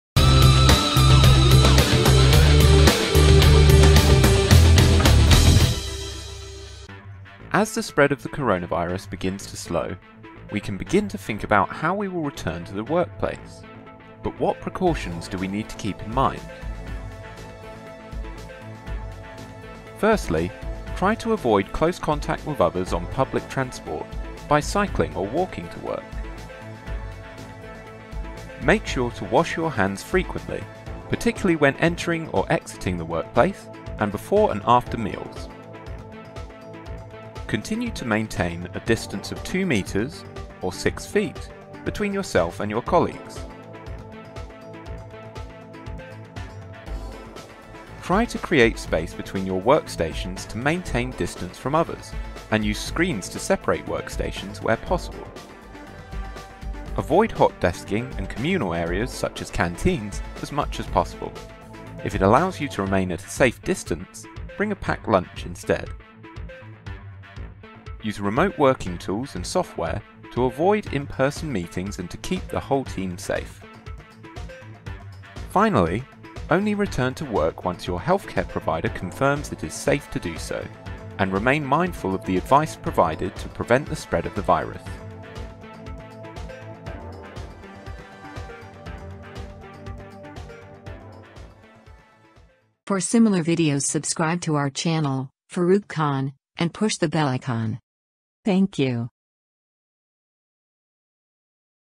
CORONA VIRUS WORK PLACE PRECAUTIONS | FAROOQ KHAN
Must Watch. As Lock-down ended, offices are opened.

Created By Farooq Khan
Development Professional | Author Writer | Poet| Dreamer & Achiever
To prevent the spread of COVID-19:
Wear a mask when physical distancing is not possible.
If you have a fever, cough and difficulty breathing, seek medical attention.
Calling in advance allows your healthcare provider to quickly direct you to the right health facility. This protects you, and prevents the spread of viruses and other infections.
Masks
Masks can help prevent the spread of the virus from the person wearing the mask to others. Masks alone do not protect against COVID-19, and should be combined with physical distancing and hand hygiene. Follow the advice provided by your local health authority.

Safe use of alcohol-based hand sanitizers
To protect yourself and others against COVID-19, clean your hands frequently and thoroughly. Use alcohol-based hand sanitizer or wash your hands with soap and water. If you use an alcohol-based hand sanitizer, make sure you use and store it carefully.

Keep alcohol-based hand sanitizers out of children’s reach. Teach them how to apply the sanitizer and monitor its use.
Apply a coin-sized amount on your hands. There is no need to use a large amount of the product.
Avoid touching your eyes, mouth and nose immediately after using an alcohol-based hand sanitizer, as it can cause irritation.
Hand sanitizers recommended to protect against COVID-19 are alcohol-based and therefore can be flammable. Do not use before handling fire or cooking.
Under no circumstance, drink or let children swallow an alcohol-based hand sanitizer. It can be poisonous.
Remember that washing your hands with soap and water is also effective against COVID-19